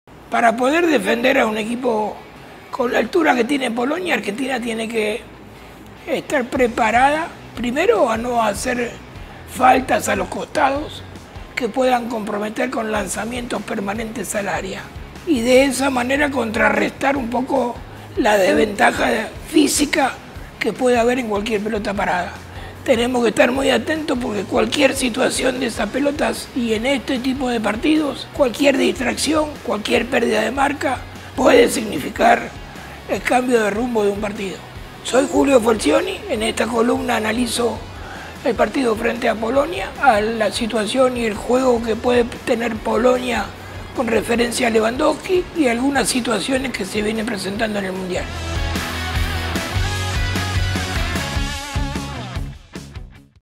POLONIA, LEWANDOWSKI y las CLAVES para que la ARGENTINA gane en Qatar | La pizarra de JULIO FALCIONI
El director técnico Julio Falcioni analizó a Polonia, el próximo rival de la Selección argentina. Debilidades, puntos fuertes y detalles a tener en cuenta del rival de la tercera fecha en la fase de grupos del Mundial de Qatar 2022.